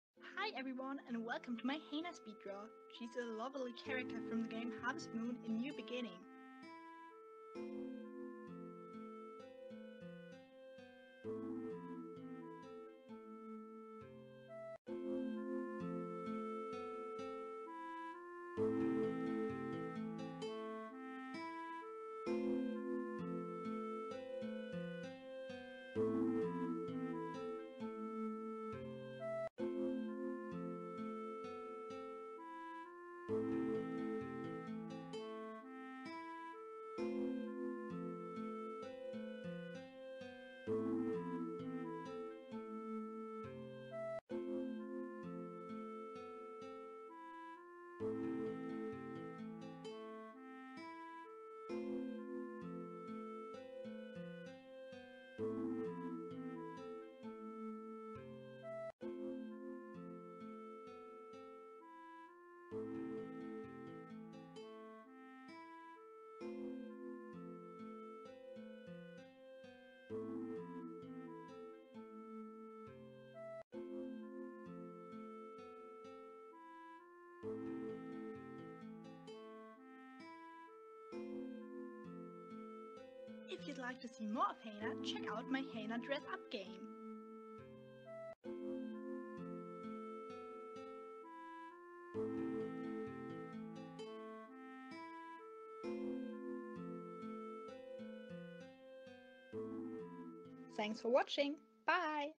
Hana Speed Draw [Harvest Moon: A New Beginning]
Hi everyone and welcome to this speed draw of Hana, a lovely character from the game Harvest Moon: A New Beginning.

Credits:
- This is merely fanart. I do not own the character or design.